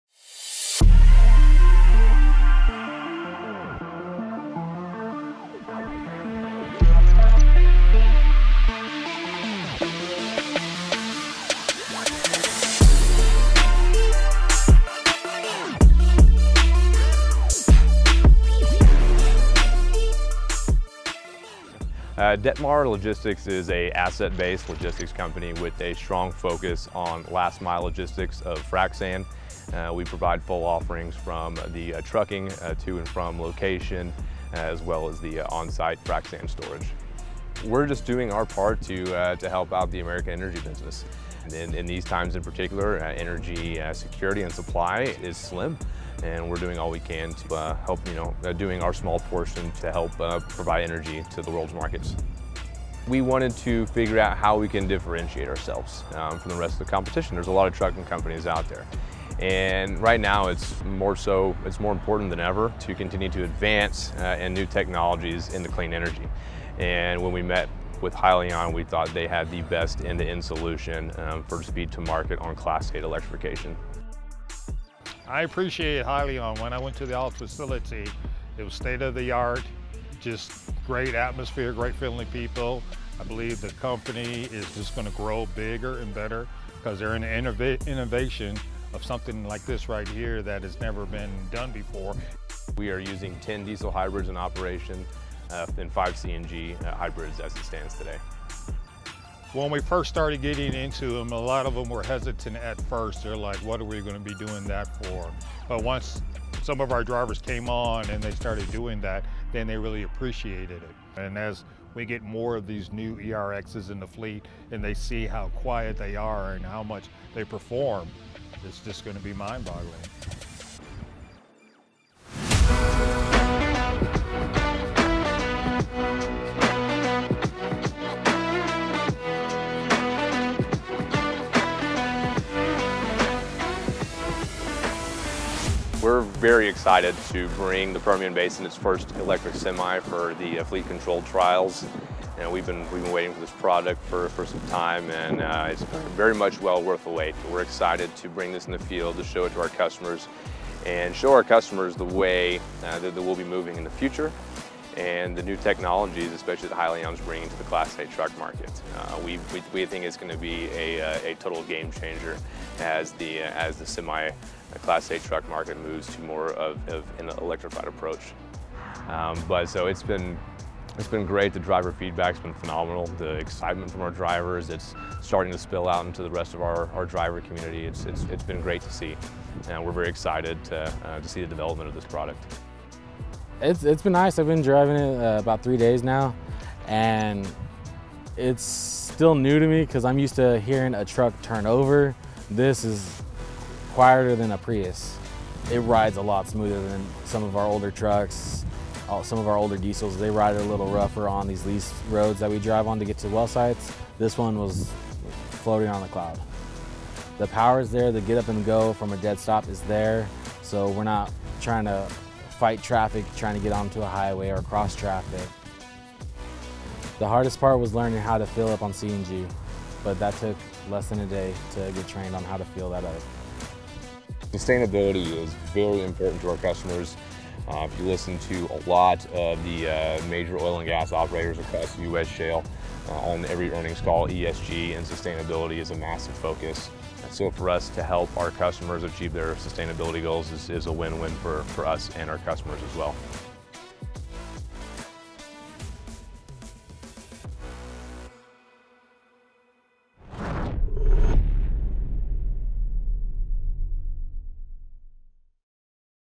Controlled Fleet Trials with the Hypertruck ERX Demo Vehicle | Detmar Logistics
Controlled Fleet Trials with the Hypertruck ERX demo vehicle continue with Detmar Logistics, Texas-based frac sand solutions provider, and early adopter of the Hyliion Hybrid technology. Detmar embedded a Hyliion Hypertruck ERX demo vehicle in its operations to experience the technology’s capabilities firsthand as it made daily hauls along West Texas routes.

With positive driver feedback and the opportunity to reach ESG goals for their customers, Detmar sees the Hypertruck ERX powertrain as an enabling technology to accelerate their growth.